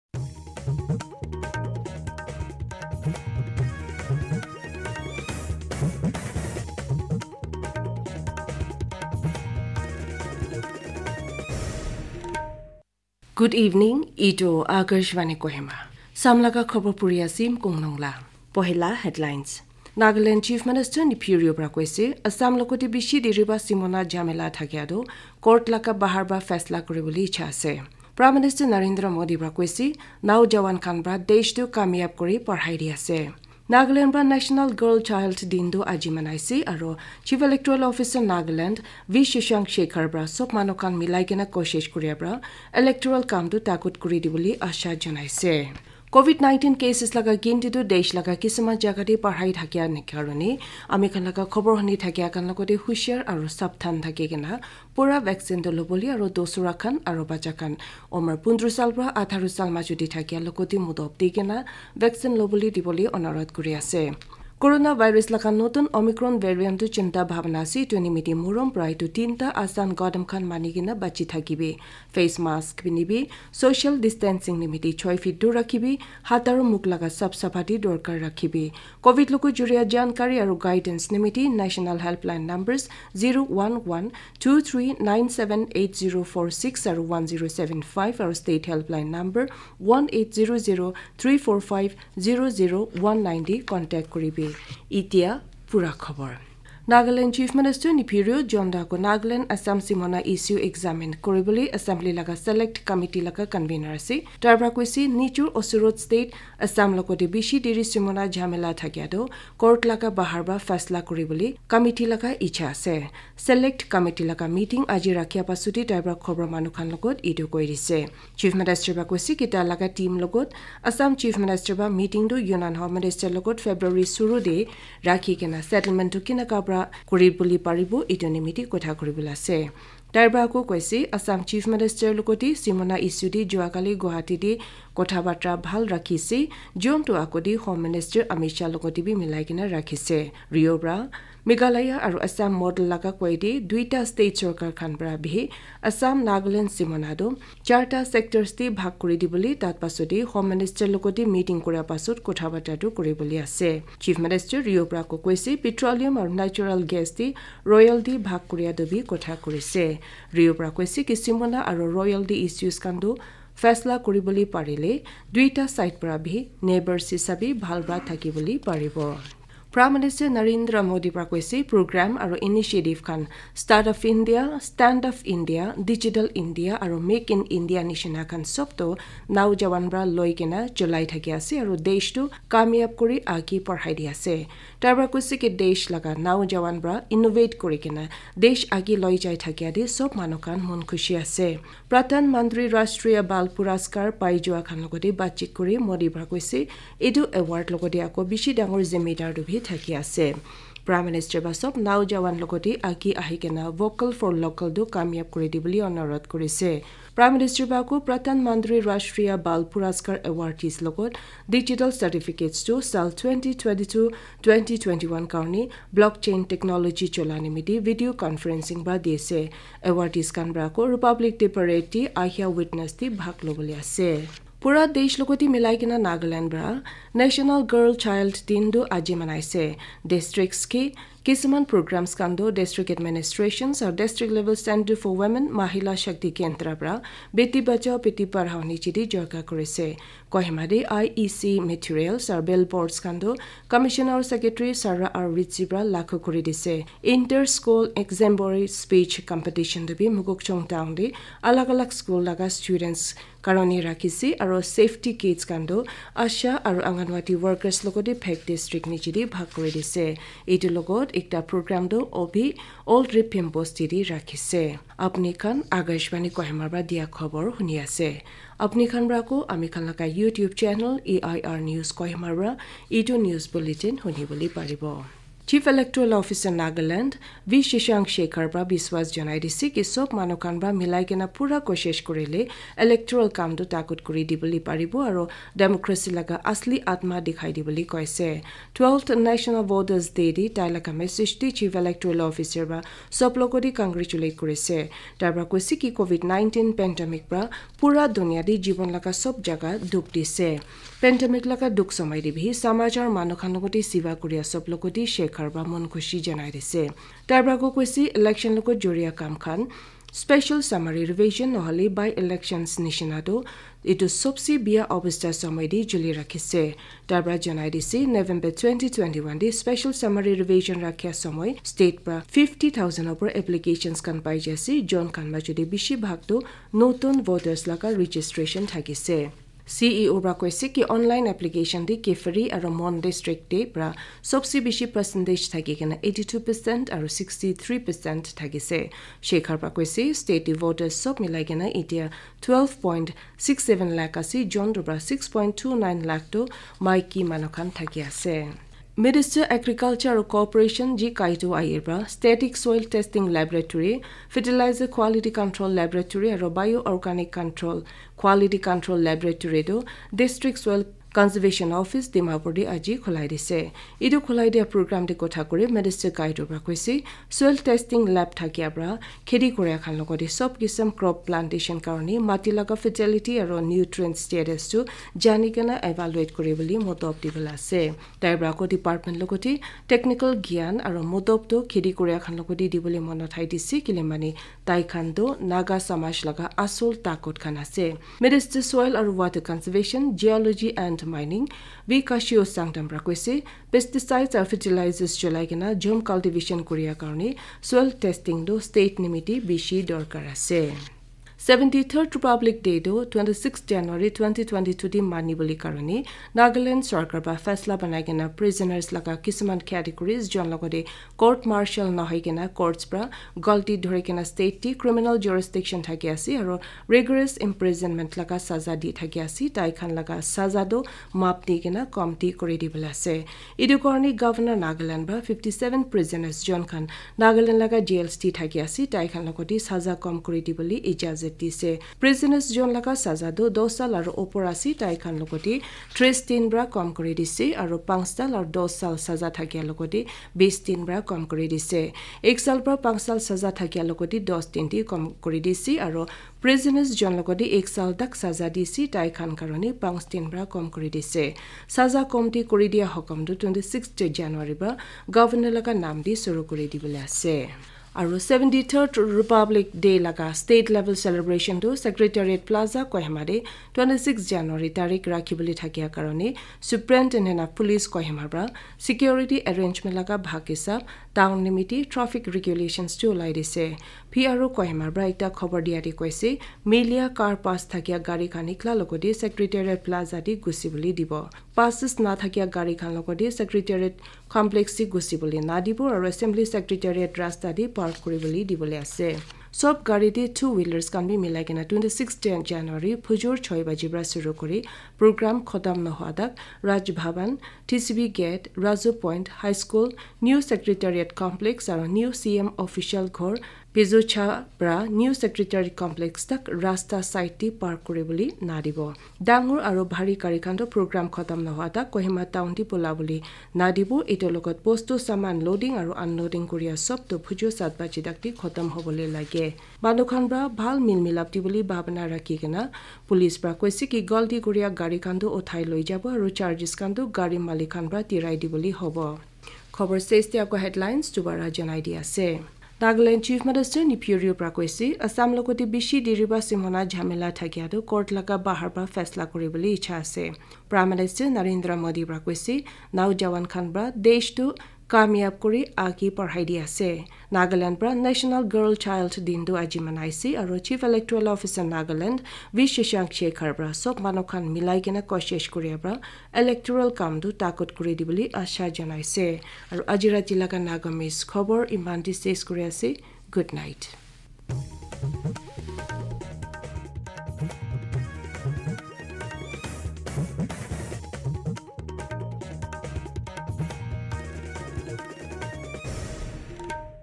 AIR News Kohima Evening Nagamese Bulletin on January 24, 2022.
:: THE HEADLINES::
• Nagaland Chief Minister, Neiphiu Rio says, it is in favor of settling the long pending boundary disputes with Assam from outside the court.

• Prime Minister Narendra Modi says young people will take the country to newer heights

• Nagaland celebrates National Girl Child day today

• And, Chief Electoral Officer Nagaland, V. Shashank Shekhar expresses hope that  every citizen will make all efforts, to strengthen the electoral process